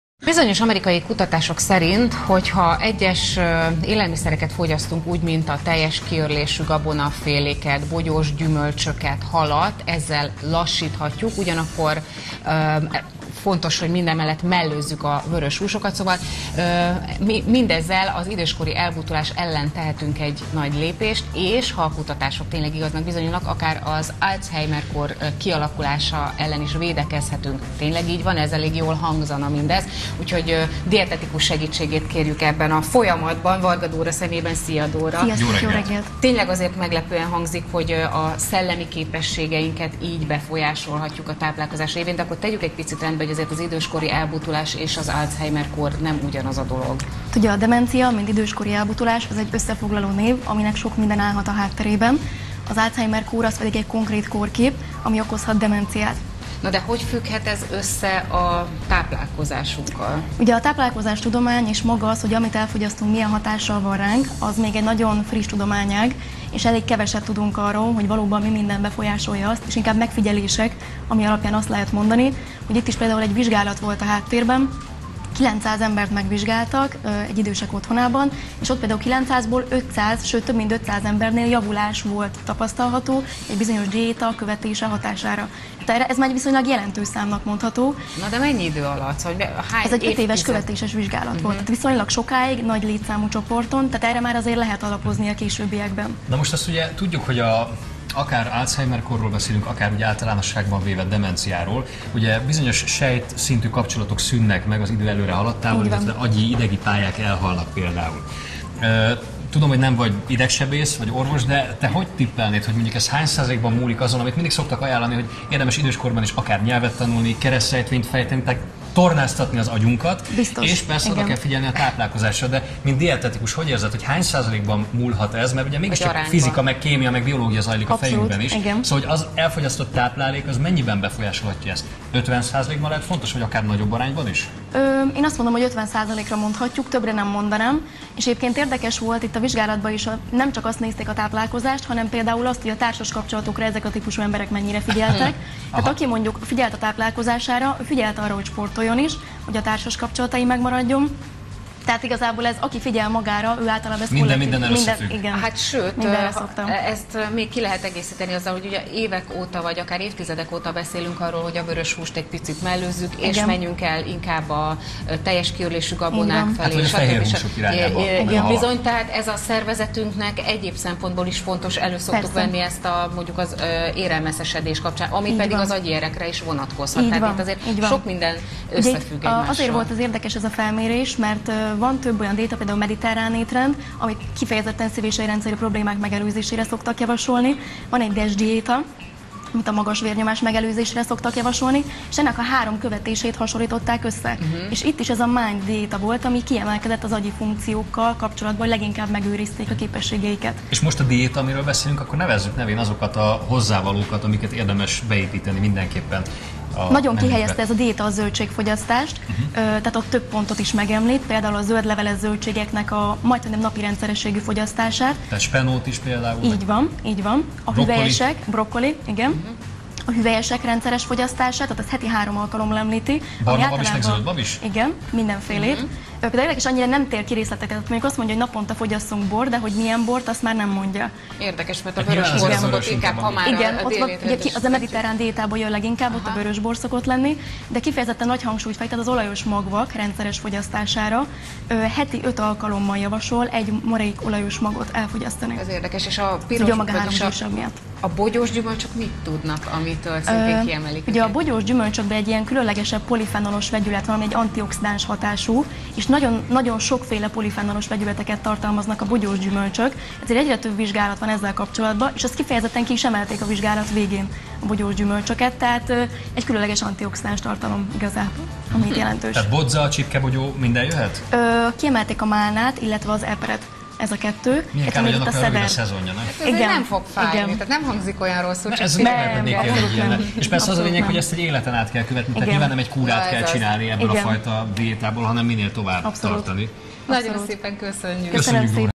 Endokrinközpont - endokrinológiáról mindent egy helyen
Egy, a napokban megjelent amerikai kutatás szerint bizonyos ételek fogyasztásával, mint például vörös húsok, bogyós gyümölcsök vagy babfélék lassítani lehet az elbutulást és az Alzheimer-kór valamint demencia kialakulásának kockázatát. Milyen bizonyítékok vannak erre vonatkozólag, tényleg hihetünk-e a tudománynak és ha igen, akkor mikor kezdjük el ezt a speciális étrendet, erről beszélgettek Varga Dóra, dietetikussal az RTL KLUB reggeli műsorában